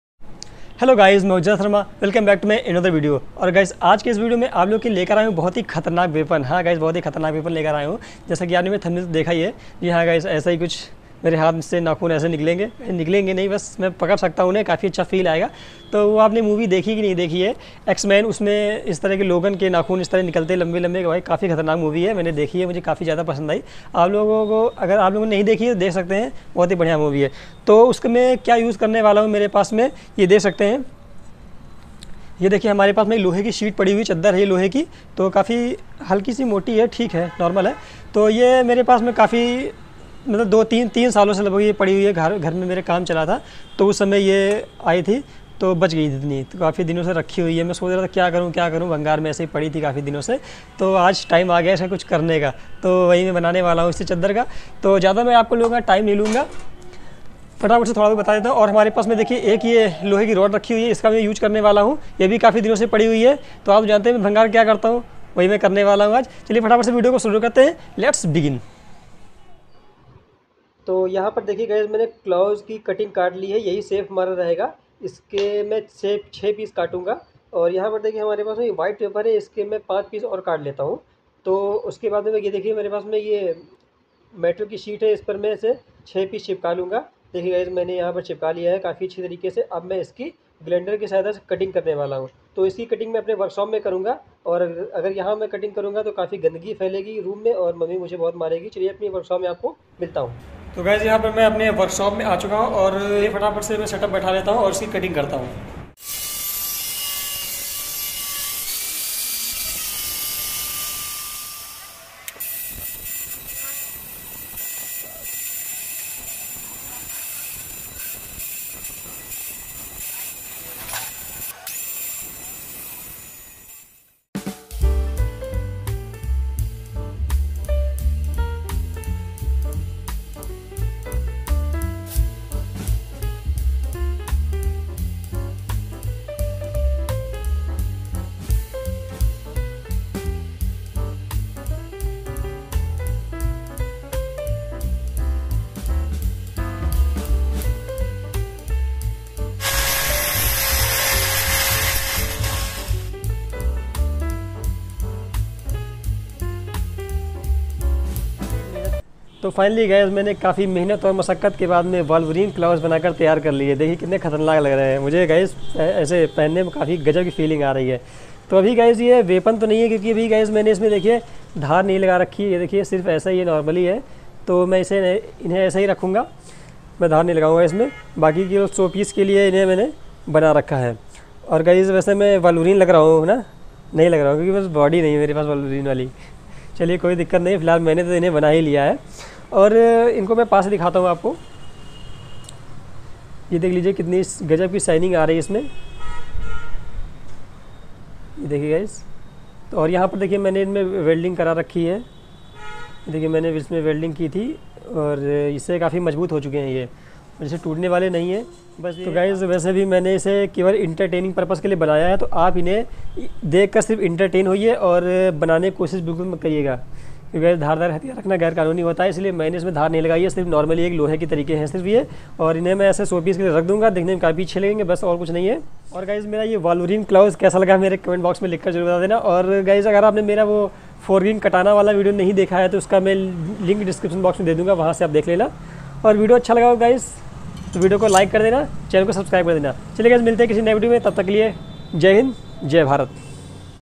How to make wolverine claws using metal | X man | craft
Discription:-

Video credit :-
 Video editor:- Jaydeep sharma
Artist:- Jaydeep sharma

How to make wolverine clows.